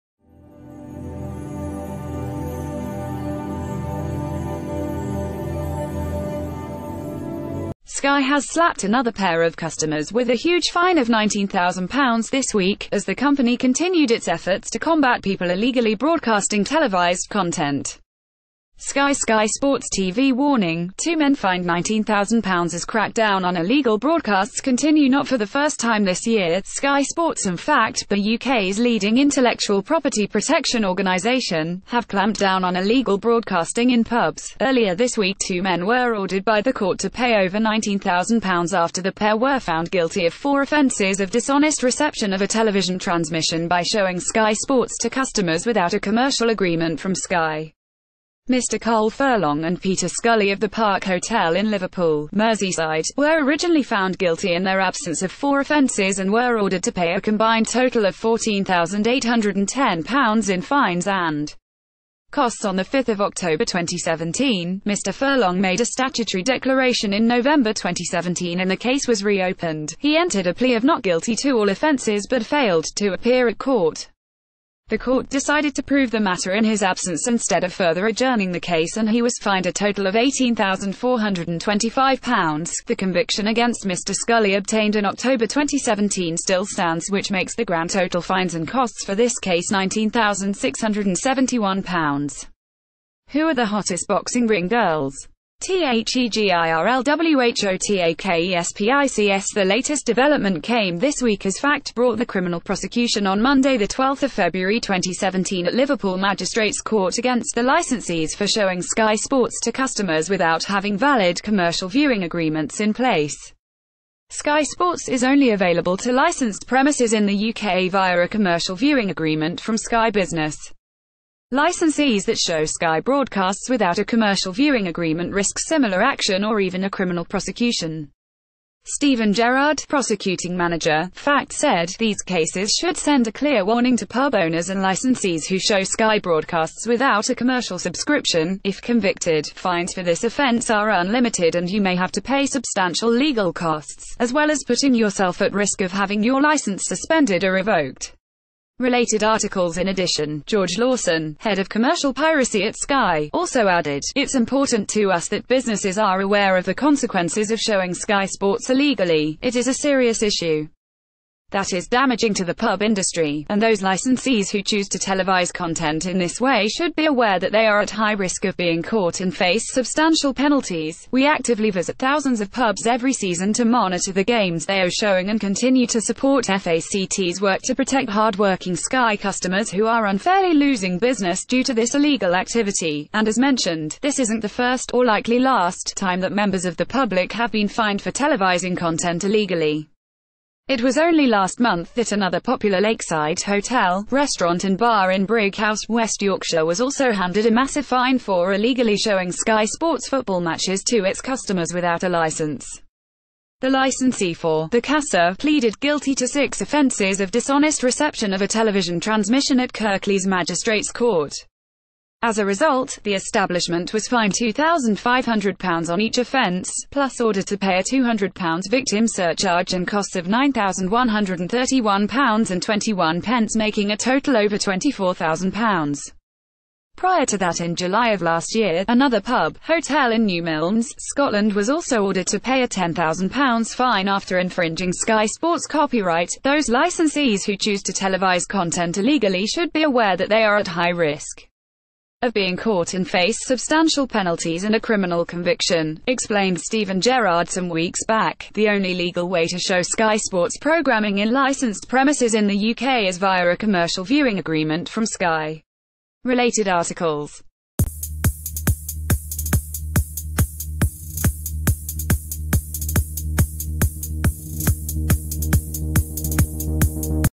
Sky Sports TV WARNING: Two men fined £19,000 as crackdown on illegal broadcasts continue
SKY has slapped another pair of customers with a HUGE fine of £19,000 this week, as the company continued its efforts to combat people illegally broadcasting televised content. SKY Sky Sports TV WARNING: Two men fined £19,000 as crackdown on illegal broadcasts continue Not for the first time this year, Sky Sports and FACT - the UK’s leading intellectual property protection organisation - have clamped down on illegal broadcasting in pubs. Earlier this week two men were ordered by the court to pay over £19,000 after the pair were found guilty of four offences of dishonest reception of a television transmission by showing Sky Sports to customers without a commercial agreement from Sky. Mr Carl Furlong and Peter Scully of the Park Hotel in Liverpool, Merseyside, were originally found guilty in their absence of four offences and were ordered to pay a combined total of £14,810 in fines and costs on 5th October 2017.Mr Furlong made a statutory declaration in November 2017 and the case was reo...